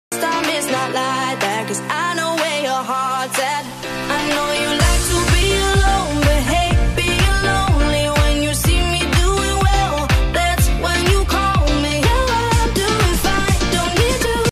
Mep Part 2 and 3 for Savanna
Kiara Breaks up with Kovu and fall in Love with Vitani😍